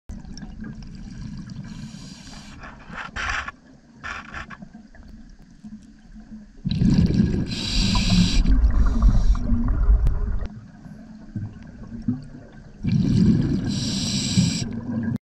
Spotted Ratfish (Hydrolagus colliei) - Tenaculum Attack!
Night diving with Ratfish (Hydrolagus colliei) San Juan County Park, San Juan Island, Washington (Salish sea).